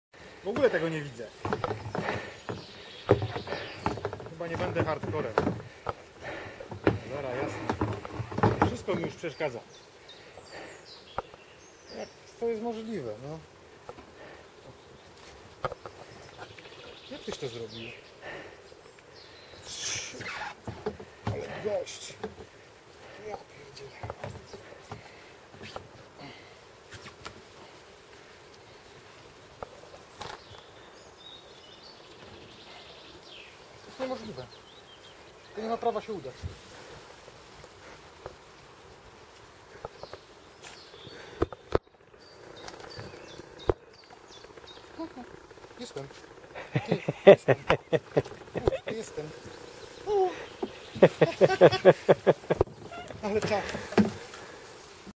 KajakiPrzemsza
trasa kajakowa na Białej Przemszy w Sławkowie przemsza.pl